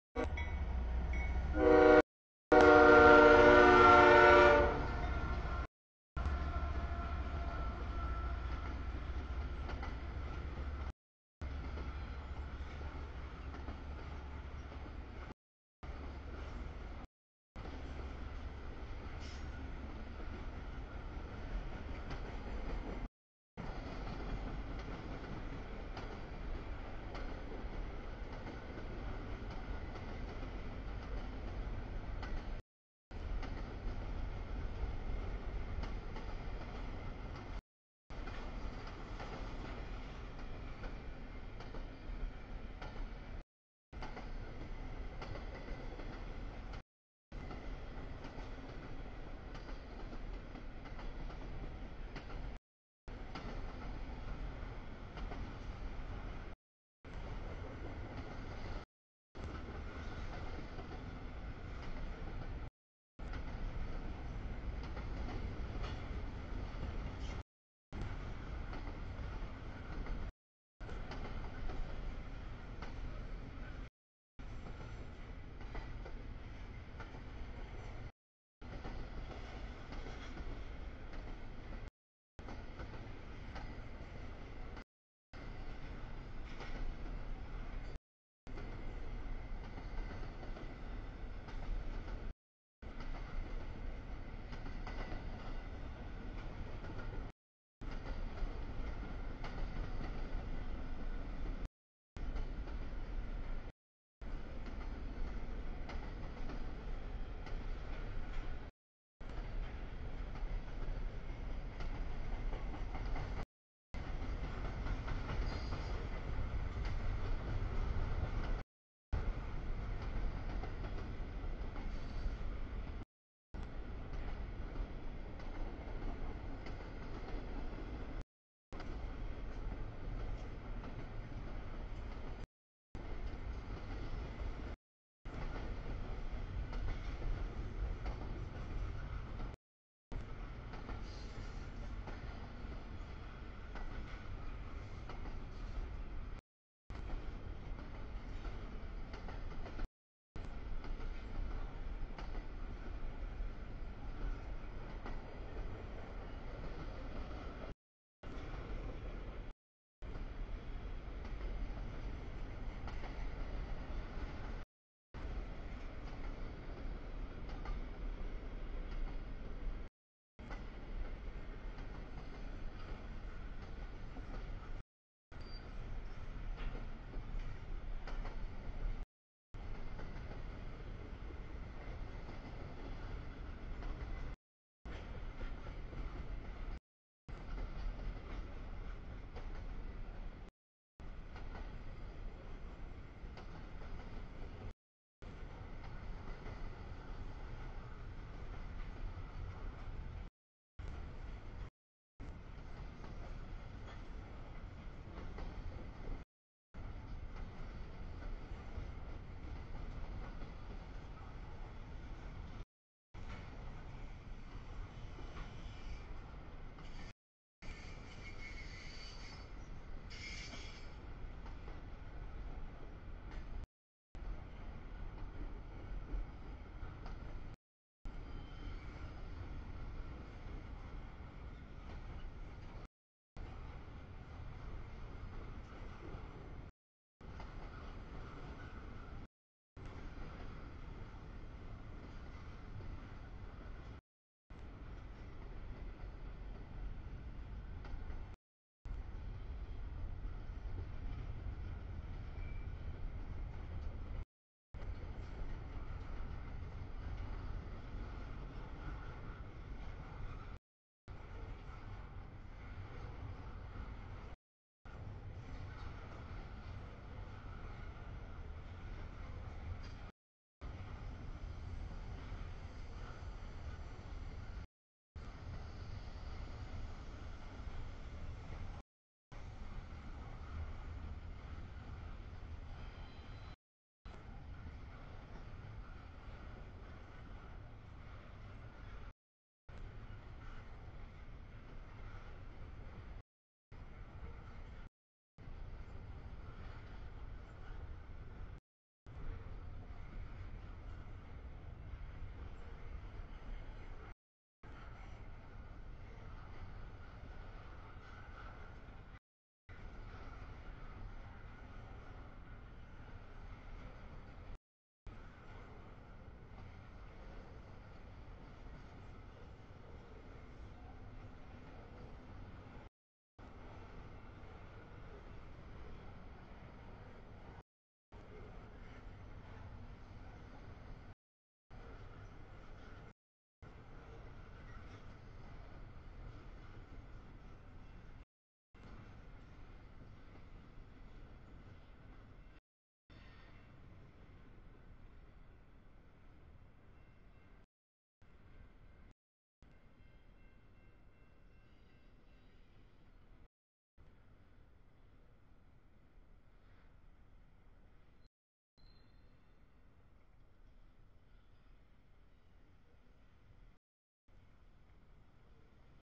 2- 15-2019 00:43 sb stack CSX 5482 Q160
00:30 sb stack CSX 3226 Q162
00:43 sb stack CSX 5482 Q160
00:55 nb stack trailers CSX 3193 Q009
01:22 nb stack CSX Q159
01:39 nb stack CSX 25 Q157
03:30 sb tanks CSX 556 K606
03:38 nb auto rack CSX Q261
06:28 sb stack trailers CSX Q010
07:04 nb stack trailers CSX 3191, 3222 Q003
07:50 sb stack trailers CSX 8006, 978 Q008
08:09 nb stack trailers CSX 5208, 3359 Q007
08:30 nb mix CSX 752, 913 Q438
09:13 sb auto rack CSX 5281 Q262
09:38 nb auto rack CSX 906, 3428 Q253
10:01 nb mix CSX 3326 DPU 3172 Q410
10:42 sb stack CSX 3270, 3371 Q158
10:49 nb mix, nysw cut, CSX 482, 6220, 3181, 8662 Q190
11:43 sb auto rack CSX 933, 3315 Q234